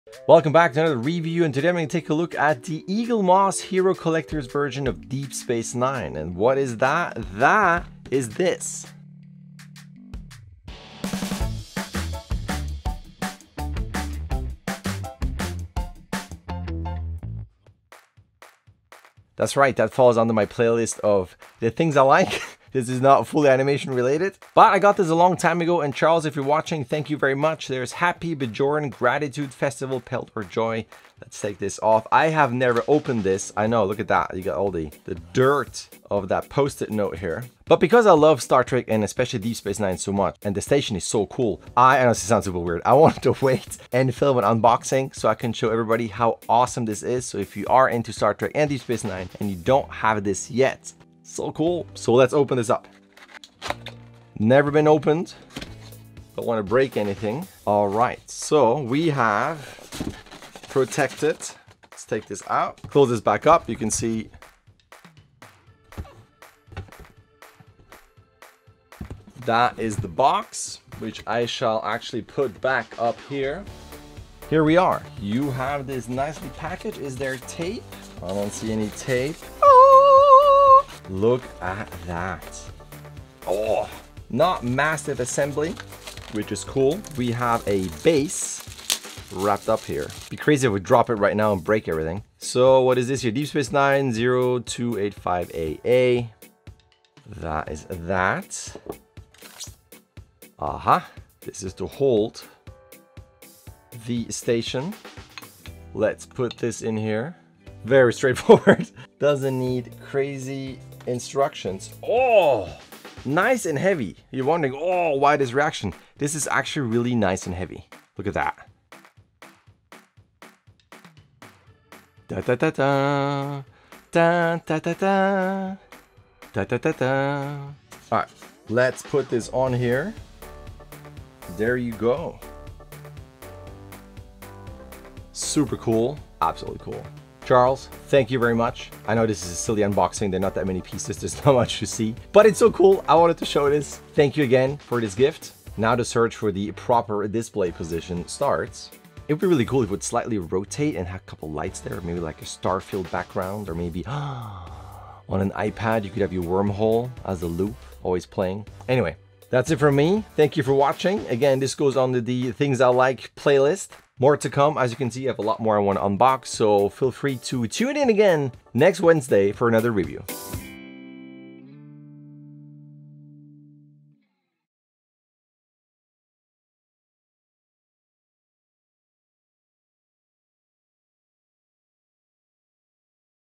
Star Trek Deep Space 9 XL Edition (Eaglemoss Hero Collector) - Unboxing
Part of the "Things I Like" playlist, let's unbox and take a look at the Star Trek Deep Space 9 XL Edition (Eaglemoss Hero Collector), because why not?! I love Star Trek and this station design has always been fascinating to me. If you haven't watcched DS9 yet you should, one of the best Trek shows out there!

Chapters:
0:00 Intro
0:20 Thank you Charles!
0:57 Unboxing
2:08 Assembled
2:39 Thanks and Subscribe!